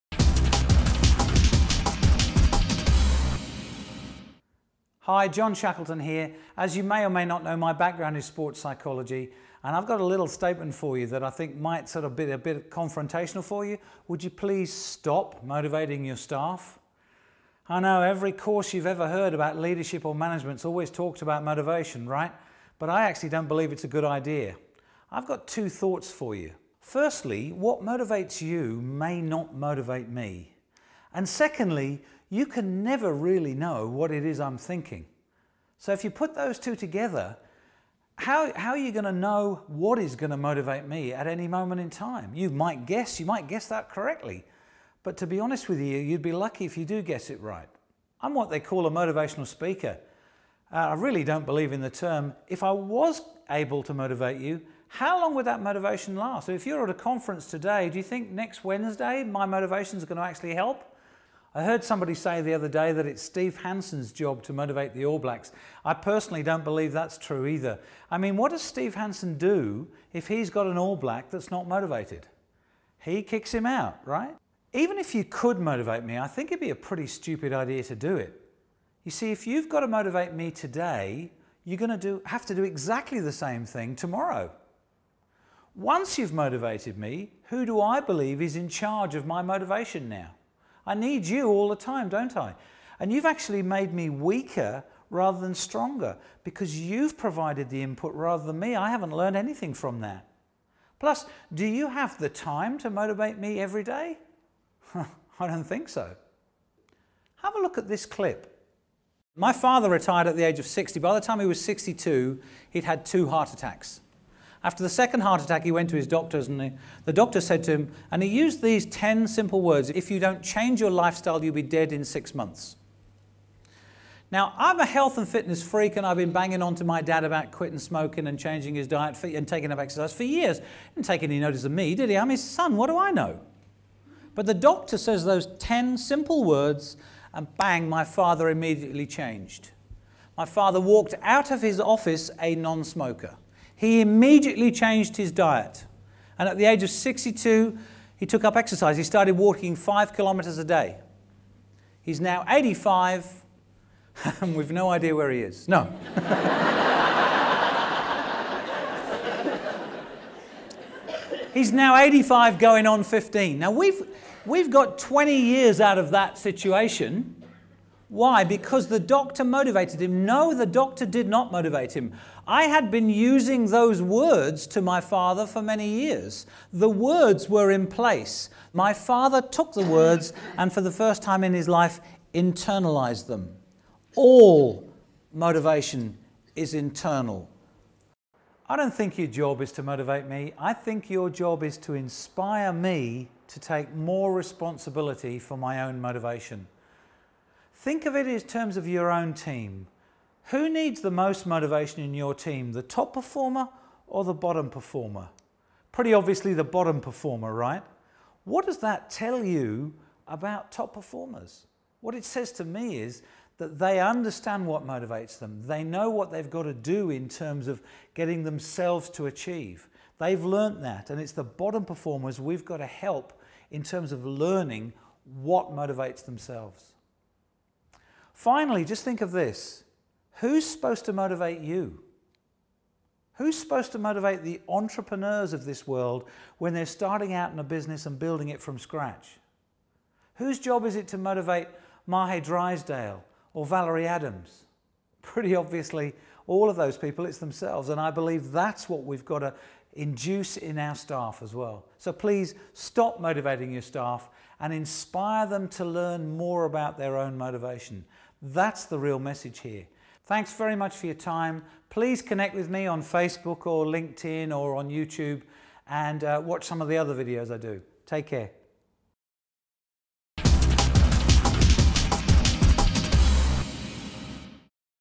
Stop motivating your staff
Even though I'm a motivational speaker I don't believe my job is to motivate people!
If I do that then I make people weaker not stronger because I've taken charge of their motivation and they've not learnt anything about motivating themselves.